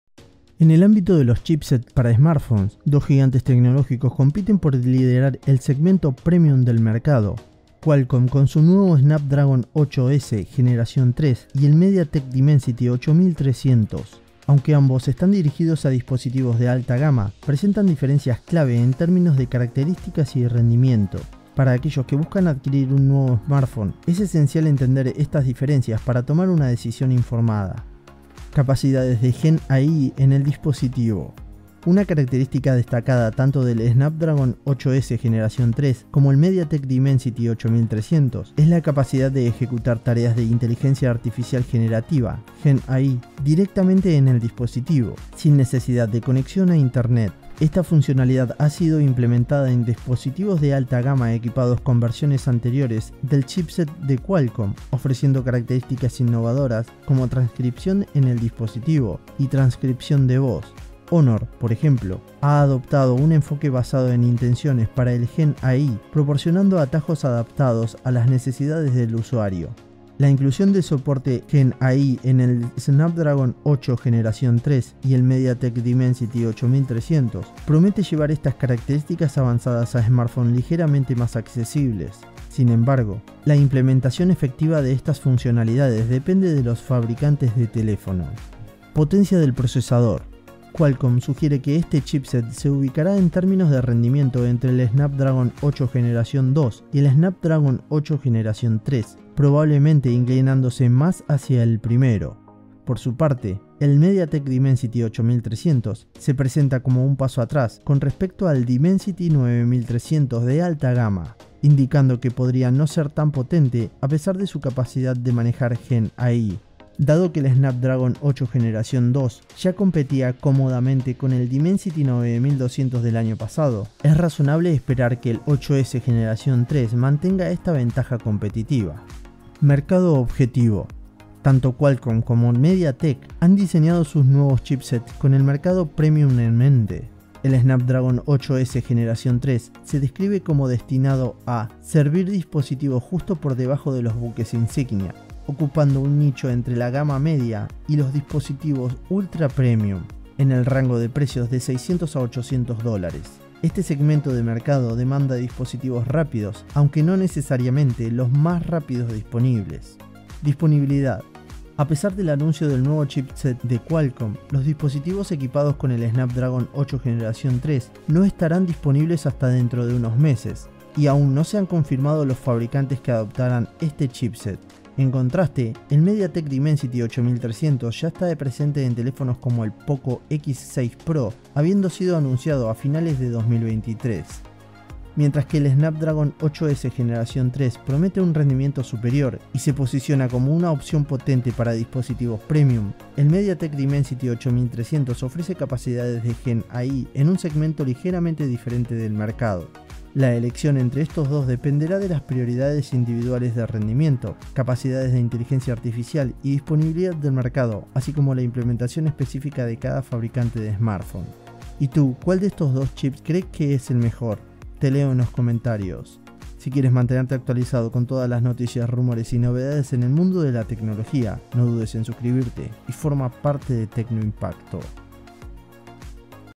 💎¿Snapdragon 8S Gen 3 vs MediaTek Dimensity 8300: Cuál Impulsa Mejor Tu Smartphone?
Descubre en este análisis detallado las diferencias clave entre dos de los chipsets más avanzados del mercado: el Snapdragon 8S Gen 3 de Qualcomm y el MediaTek Dimensity 8300. Ambos son contendientes en el segmento premium de smartphones, pero ofrecen características y rendimientos distintos que podrían influir en tu próxima decisión de compra.

Exploramos las capacidades de GenAI que permiten a estos chipsets realizar tareas avanzadas de inteligencia artificial directamente en el dispositivo. Analizamos cómo el Snapdragon 8S Gen 3 se posiciona entre los chipsets más potentes de Qualcomm y cómo el MediaTek Dimensity 8300 se compara con su predecesor y rivales.

Además, detallamos la orientación de mercado de cada chipset, con Qualcomm buscando satisfacer el segmento de gama alta no absoluto con precios entre $600 y $800, mientras que MediaTek se enfoca en una implementación más amplia en dispositivos accesibles con capacidades avanzadas de GenAI.

No te pierdas nuestras conclusiones sobre cuál de estos chipsets podría ser la mejor opción para los usuarios que valoran tanto el rendimiento como las funciones de inteligencia artificial. ¡Explora con nosotros qué chipset liderará la próxima generación de smartphones!